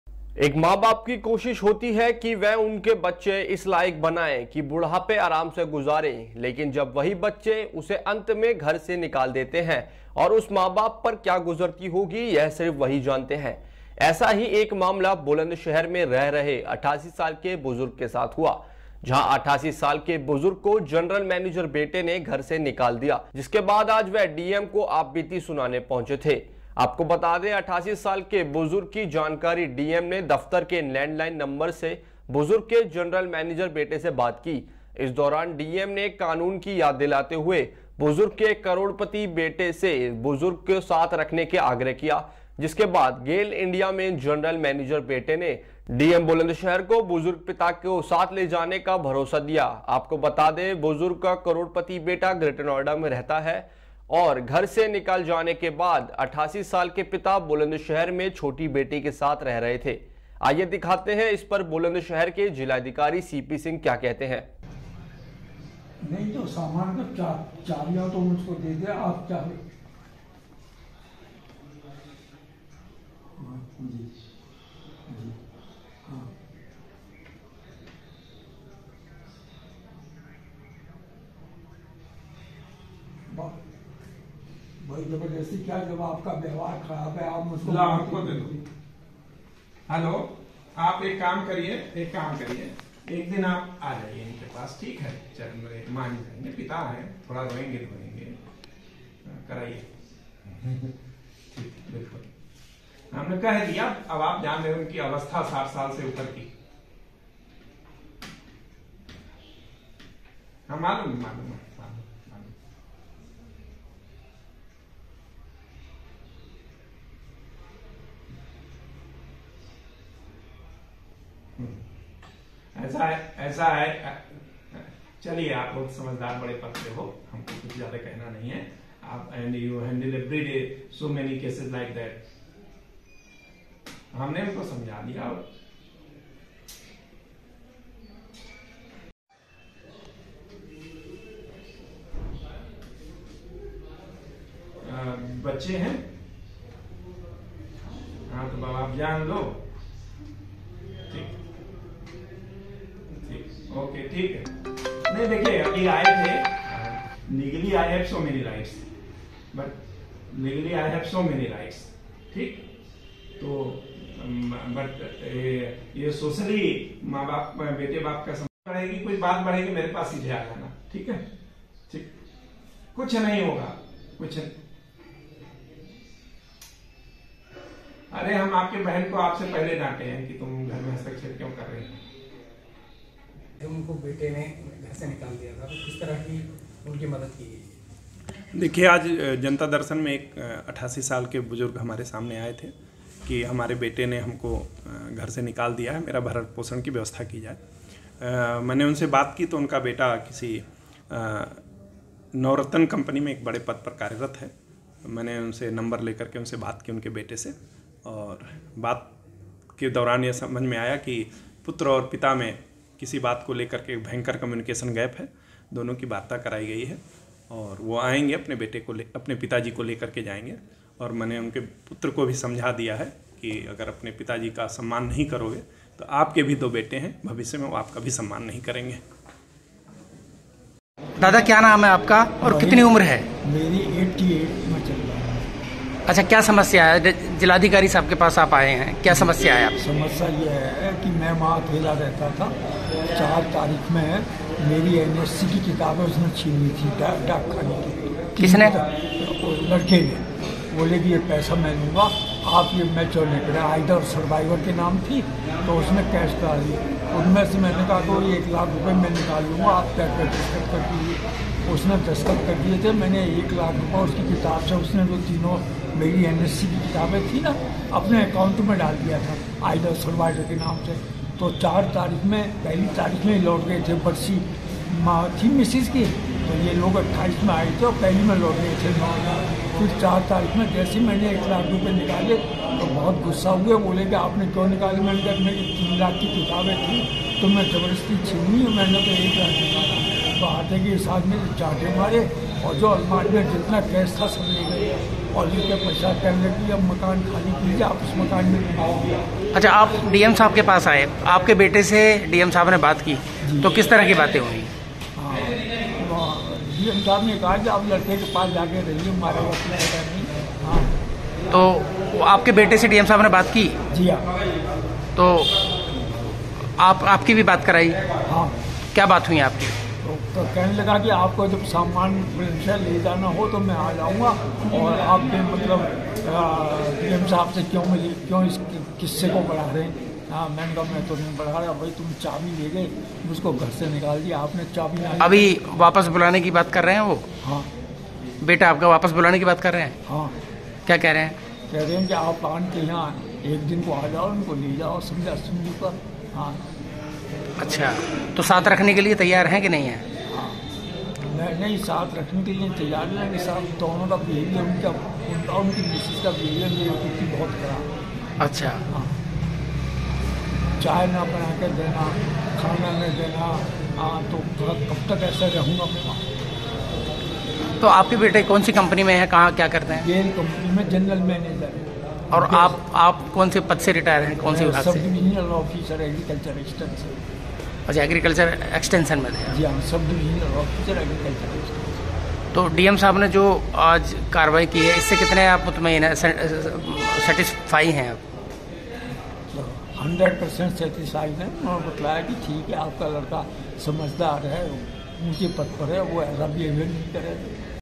Bulandshahr: जनरल मैनेजर बेटे ने 88 साल के पिता को नोएडा के घर से निकाला, डीएम ने मिलवाया।
= General manager son threw out 88 year old father from Noida house, DM introduced him.